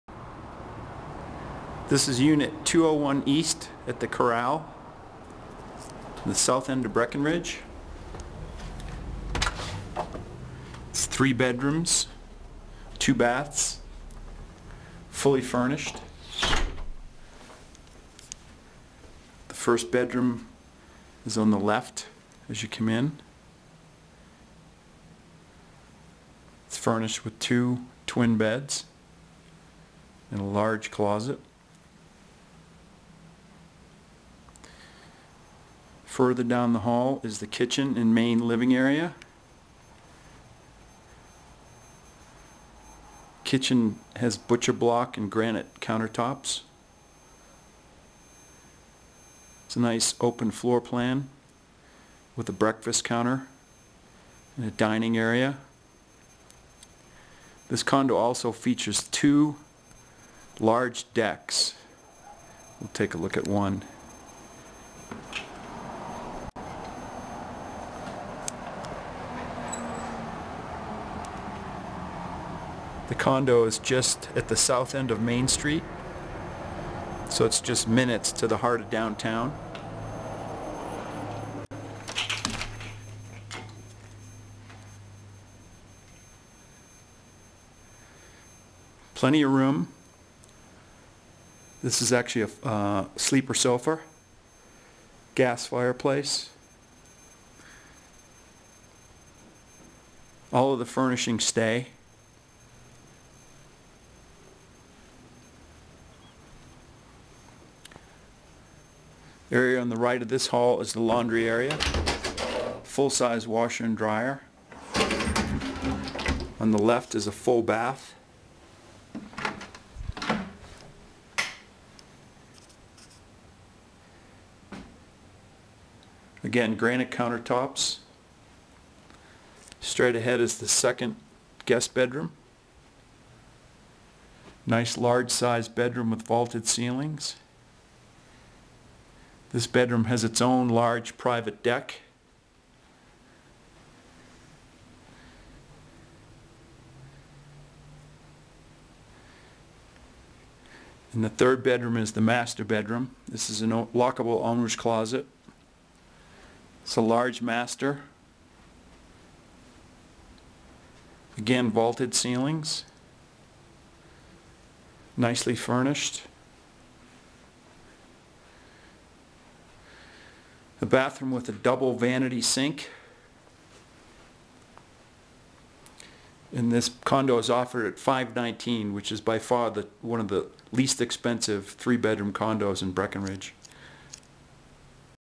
Corral at Breckenridge
Walk to the Quicksilver Lift from this large three bedroom in-town residence.  Priced below market, with over 1,400 square feet and two large decks, there's plenty of room.  The dues includes heat and lots of amenities like high speed internet, steam sauna, hot tub, and workout facility.  This condo also has a private one car garage witha locakable owners closet.  the Corral recently completed a major renovation.